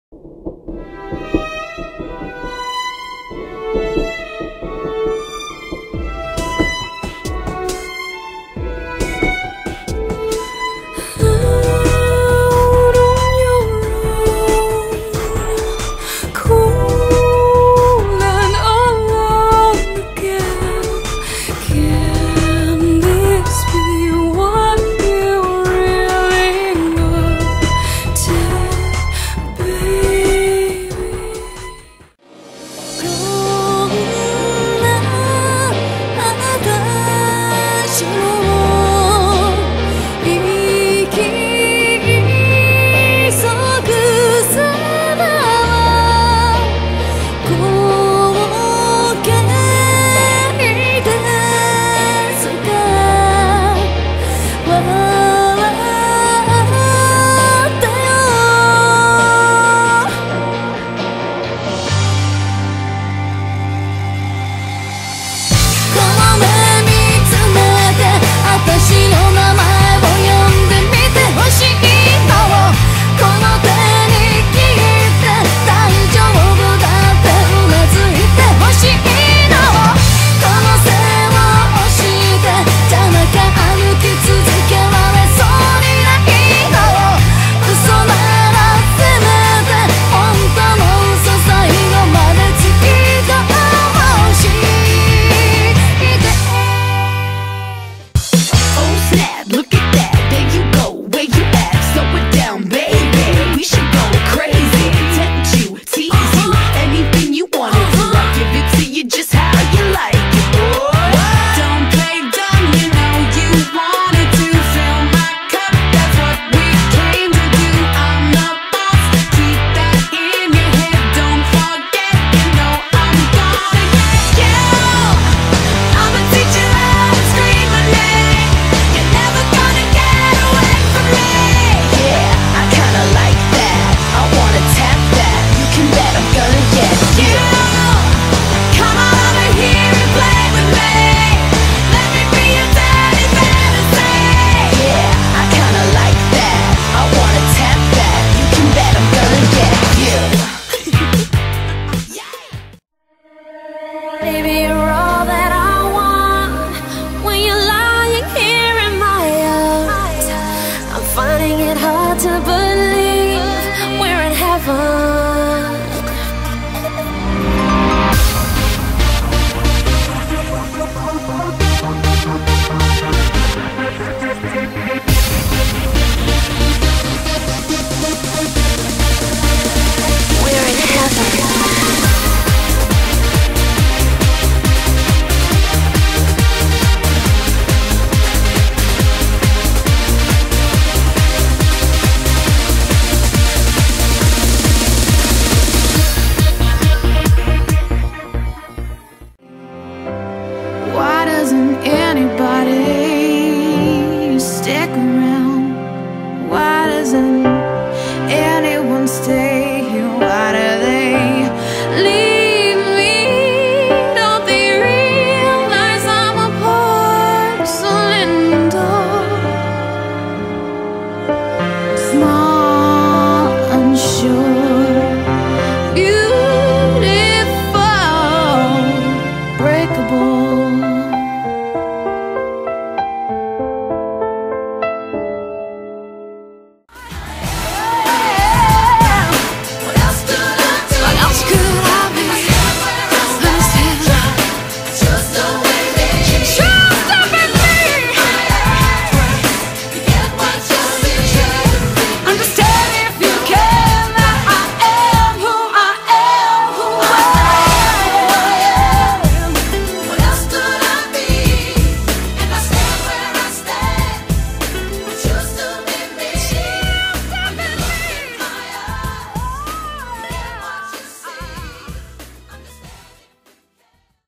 I Am project
My digital I Am project for my EDUC-419 class. Background music clips from Evanescence, Ayumi Hamasaki, Megan McCauley, DJ Sammy, and Lara Fabian.